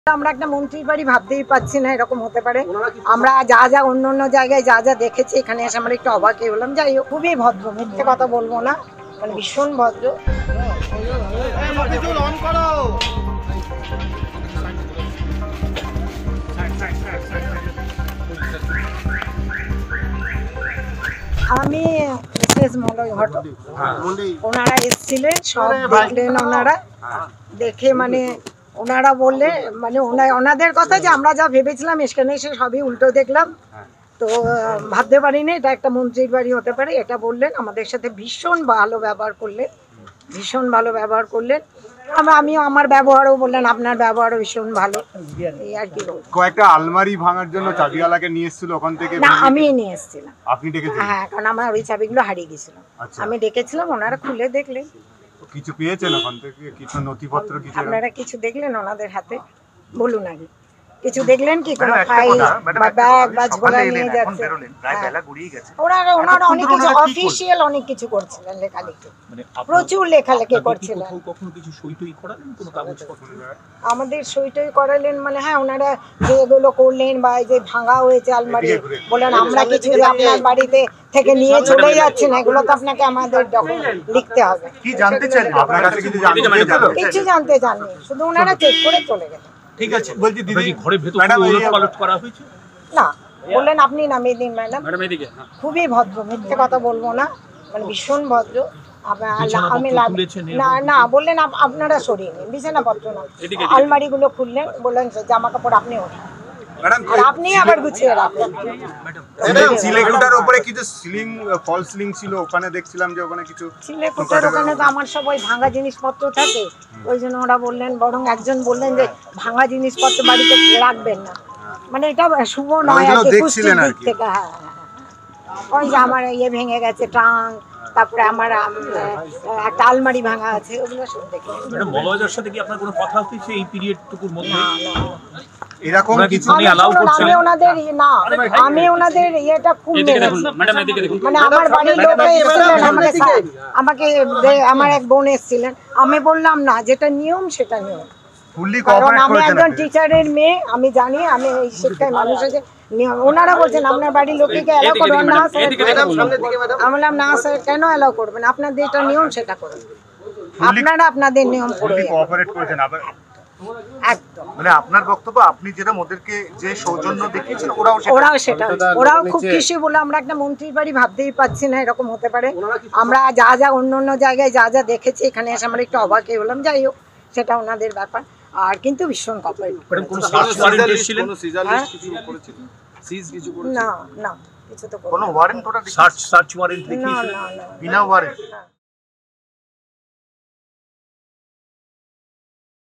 Malay Ghatak: 'CBI-এর ভীষণ ভালো ব্যবহার' বললেন মলয় ঘটকের স্ত্রী| Malay Ghatak Wife| Ei Samay
সংবাদ মাধ্যমের মুখোমুখি হন মলয় ঘটকের স্ত্রী সুদেষ্ণা ঘটক। প্রায় চার ঘণ্টা তল্লাশি। এই প্রসঙ্গে প্রশ্ন করা হলে সুদেষ্ণা ঘটক বলেন, “ওঁরা এসে সব দেখলেন। বললেন, ভাবতে পারিনি এটা মন্ত্রীর বাড়ি হতে পারে। আমাদের সঙ্গে ভীষণ ভালো ব্যবহার করলেন। শুধু তাই নয়, বললেন আমার ব্যবহার ভীষণ ভালো।” চাবিওয়ালার ঘরের ভেতরে প্রবেশ প্রসঙ্গে সুদেষ্ণা ঘটক বলেন, “চাবিওয়ালাকে আমি ডেকেছিলাম। কারণ, কিছু আলমারির চাবি হারিয়ে গিয়েছিল। ওই নতুন চাবি দিয়ে আলমারি খুললাম।” মলয় ঘটকের বাড়ি থেকে কী কিছু নিয়ে গেলেন তদন্তকারীরা?